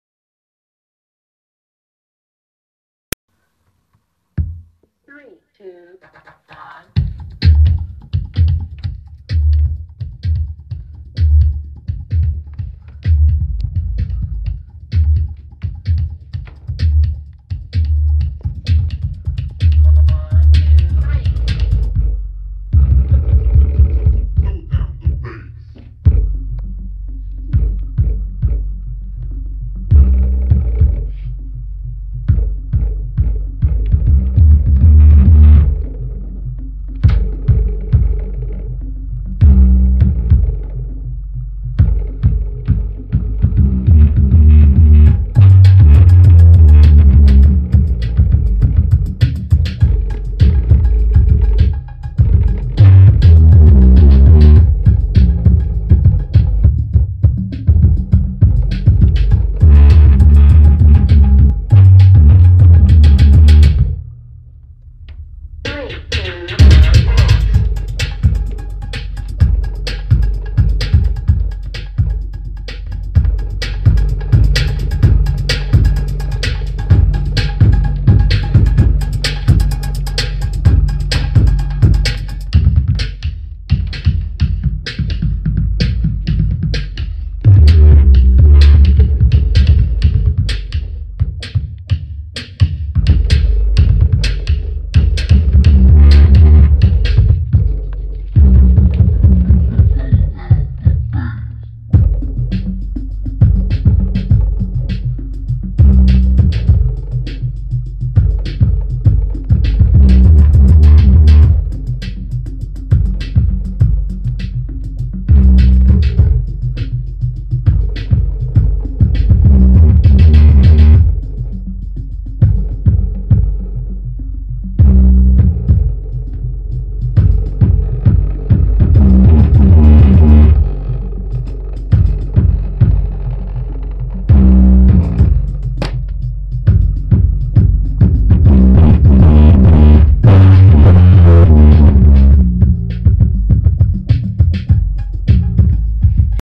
Bass test LOL
My subwoofer  in my room only at 70% , couldn't put louder lol , soon will have another video of it at full ! When nobody will be home xD / SONY SUPER BASS